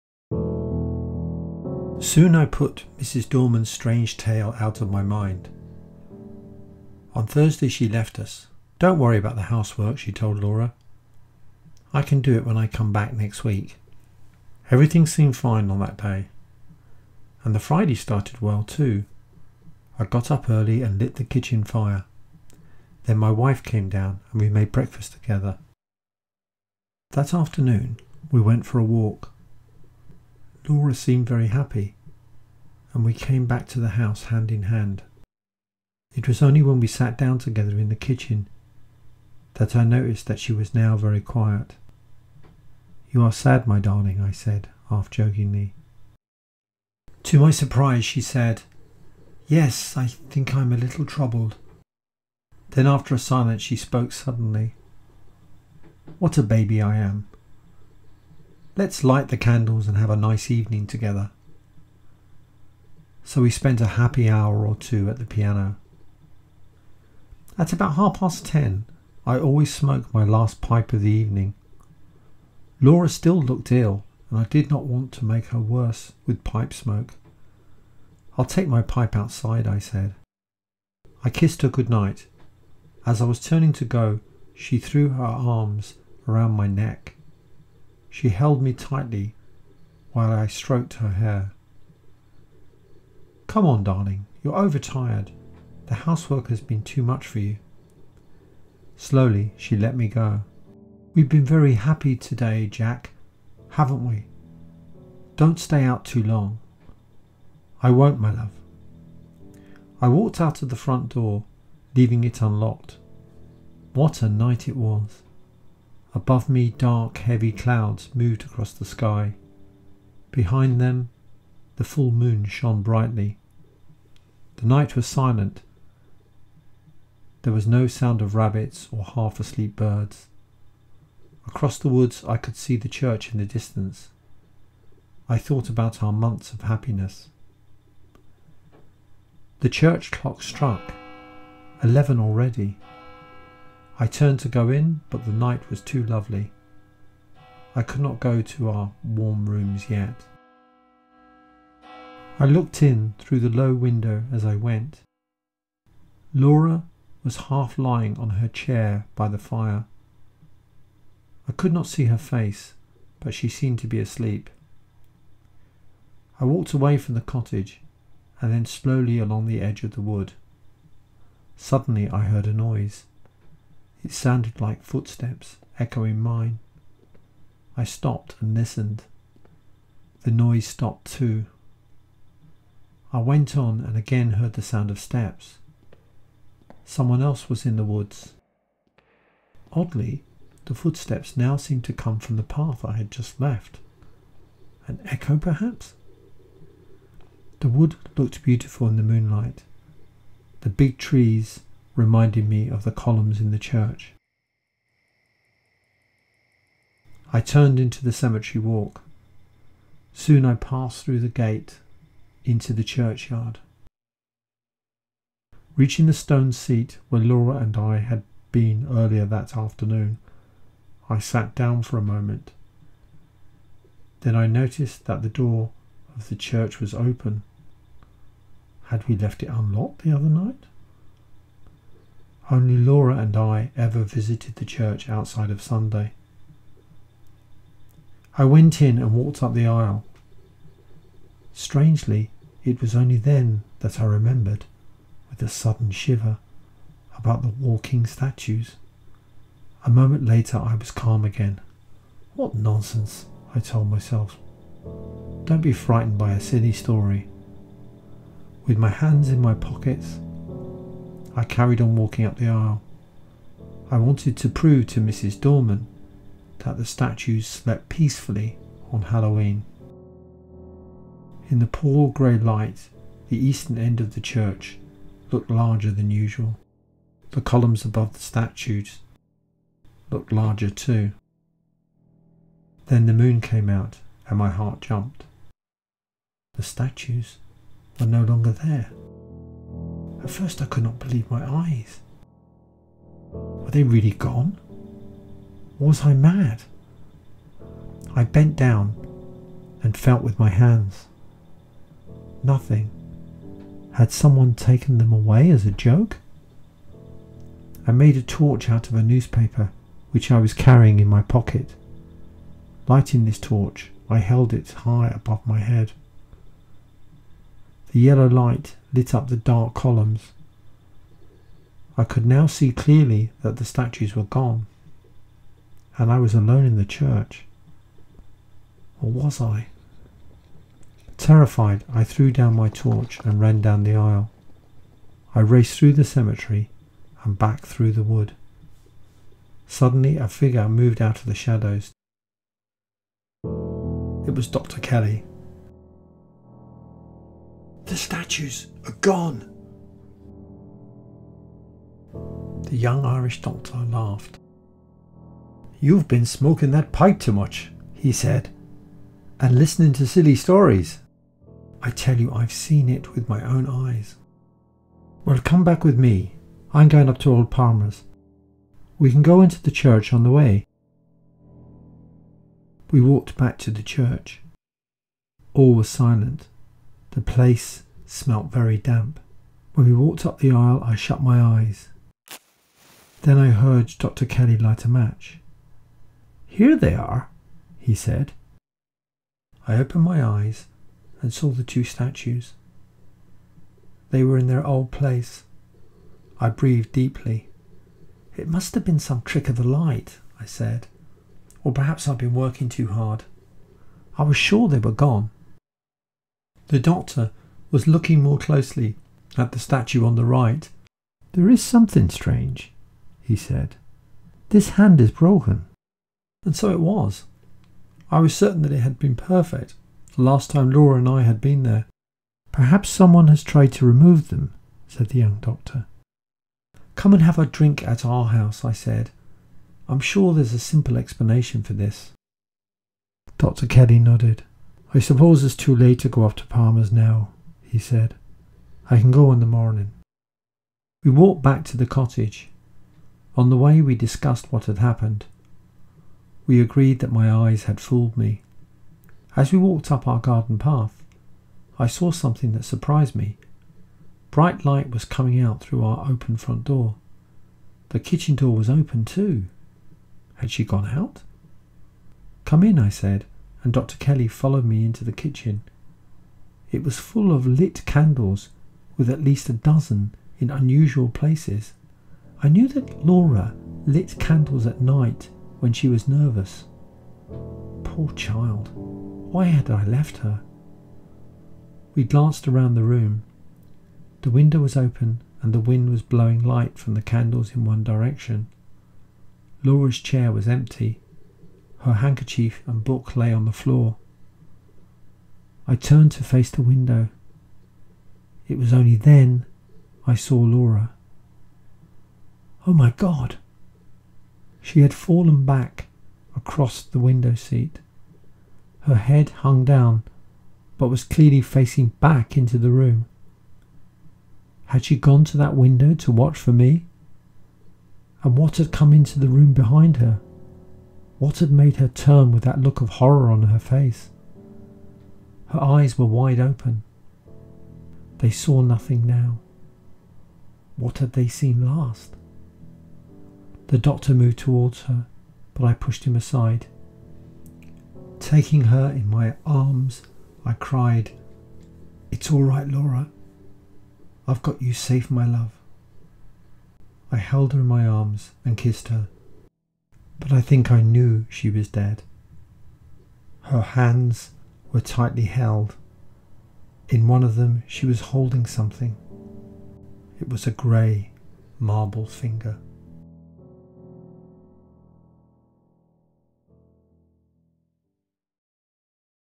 Man Size in Marble by E Nesbit - Part Two
Concluding part of classic ghost/horror story by the author of The Railway Children - see part one

The couple visit the church on the afternoon of Halloween.

Retold for audio in two part complete version (under 15 minutes). Complete adapted and original texts free to access

Ideal for classroom use with young learners and/or ESOL/EFL/ESL students.